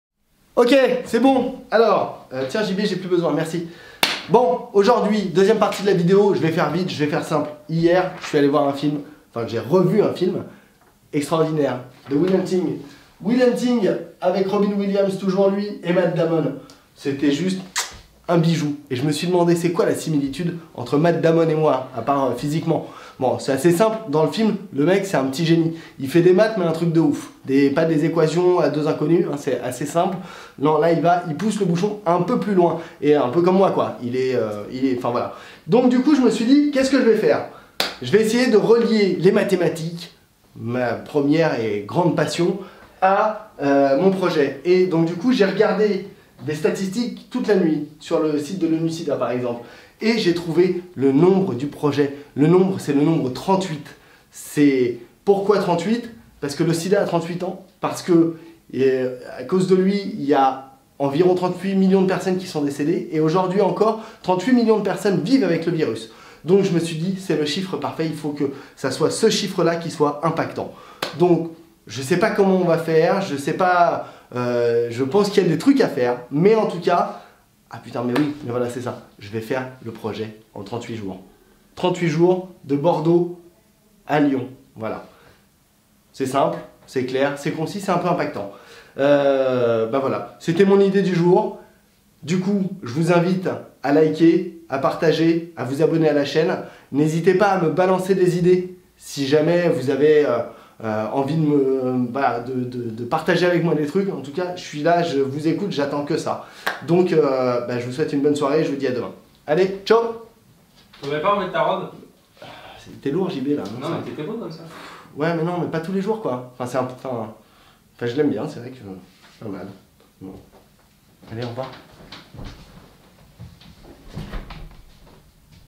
VLOG #07 - J'ai Une Idée... je pars de Bordeaux (partie 2)
J'ai continué mes recherches sur le Fonds Mondial et sur le financement de la lutte contre le sida. 38 millions de morts, 38 millions de personnes qui vivent avec le VIH... on peut en finir, il faut vraiment qu'on réveille un peu tout le monde. Je sens que mon idée commence à prendre vraiment forme. C'est le moment de me dire si vous avez d'autres pistes !
Youtube, c'est une aventure pour moi... chaque jour, j'essaye de vous partager une idée nouvelle. On va voir où ça nous mène. Vous en pensez quoi ?